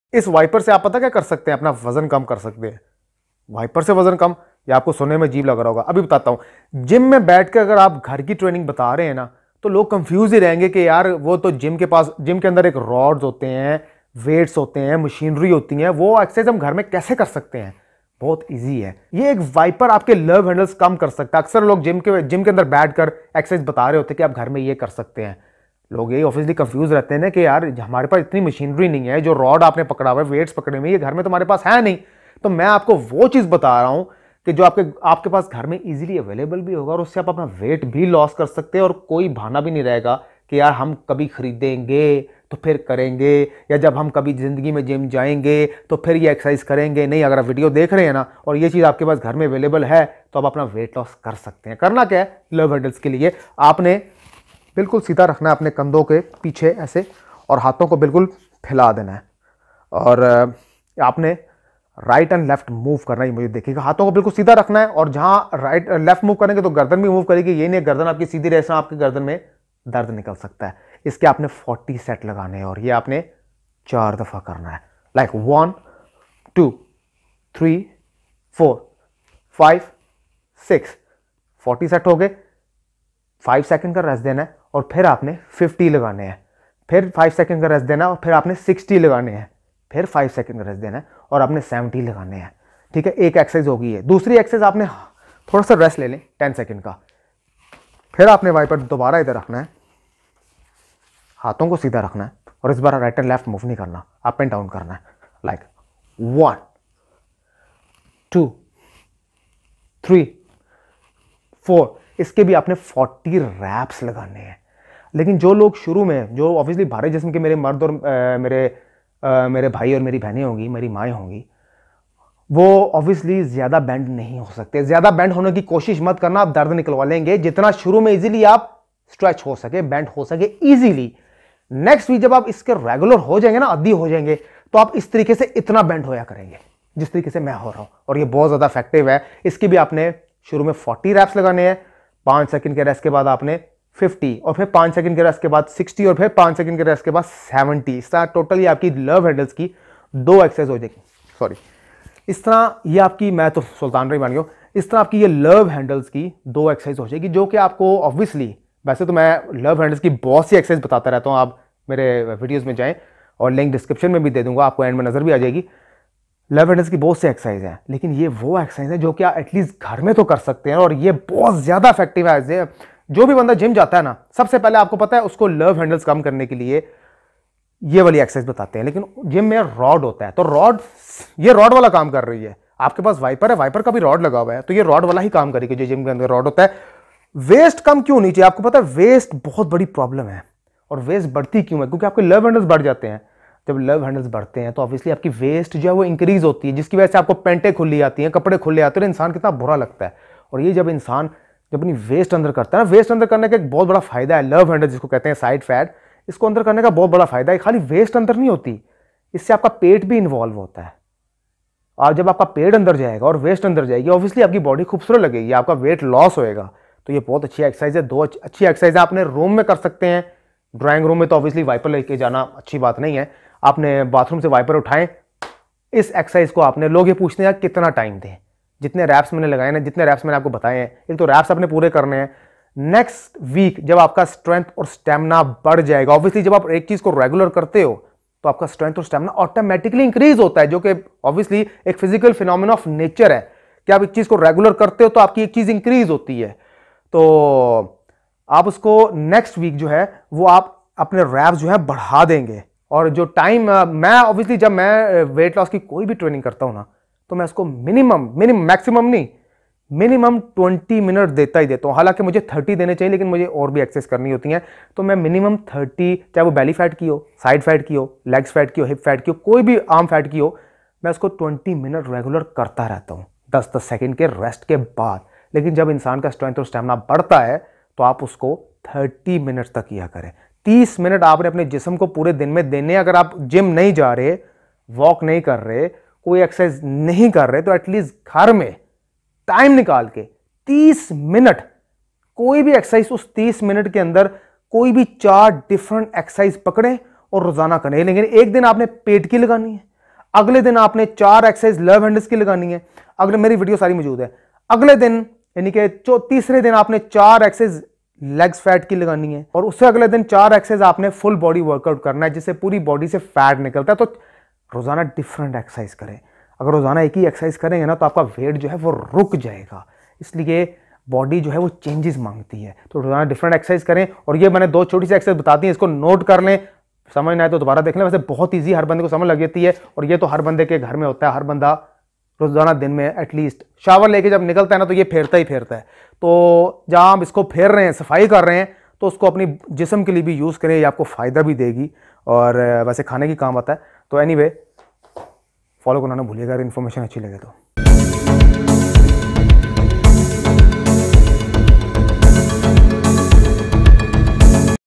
Reduce side fat at home | reduce love handles in 7 days | Urdu/Hindi
love handles reduce exercise
In this video, I have shared 1 Simple Tips To Lose Love Handles Fast.
Especially for those people who wanted to get rid of LOVE HANDLES/SIDE FAT.

Try these exercises daily with EMPTY STOMACH.
Remember, Diet plays an important role.

Cheap Diet Plan | Fat Loss Diet Plan | Low Budget Diet for Weight Loss | Urdu/Hindi